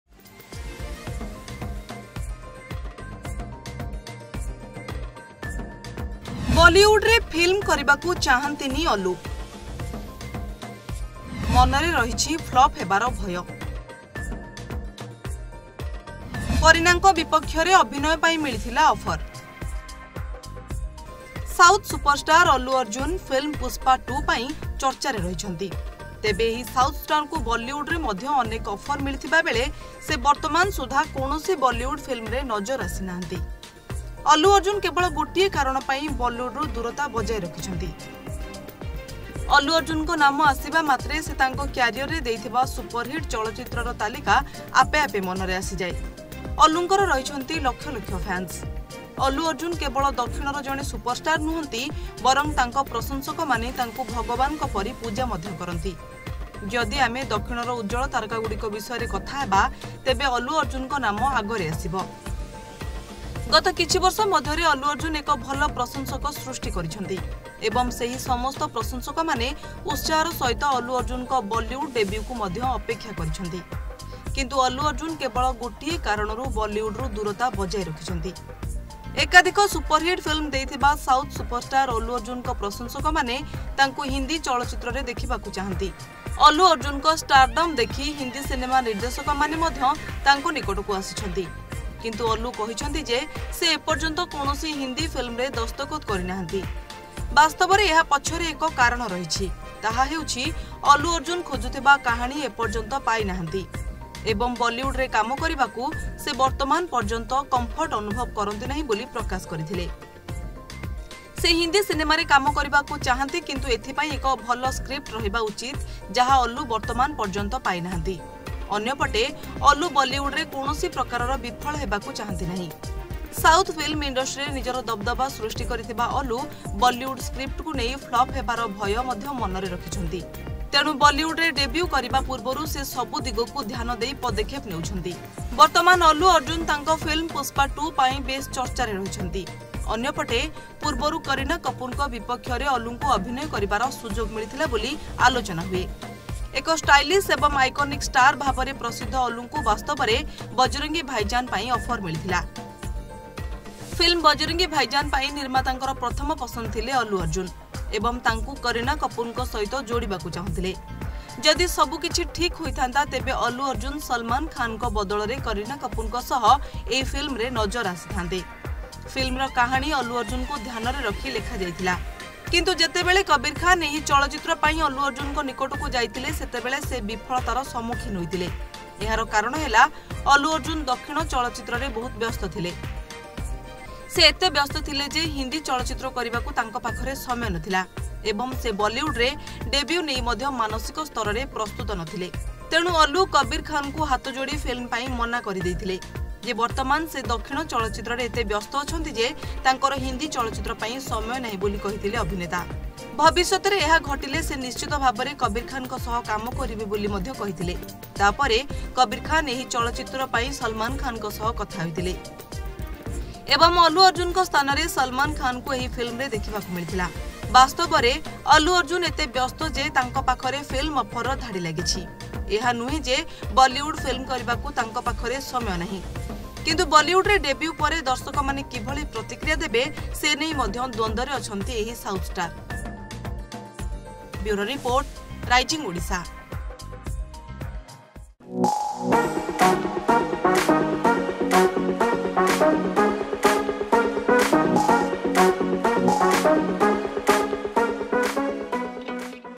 Allu Arjun Bollywood Debut | Allu Arjun Movies Will Flop If He Works in Bollywood! | Tollywood News
Tollywood Star Allu Arjun Movies Will Flop If He Works in Bollywood!